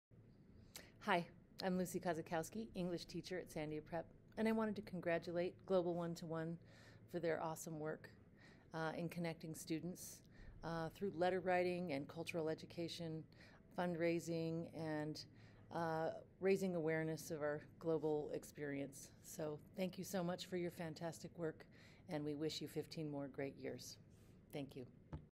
Lucy Kozikowski in New Mexico (Teacher)
Lucy Kozikowski is a teacher in Albuquerque, New Mexico whose students have participated in Global One to One virtual exchange programs for 6+ years!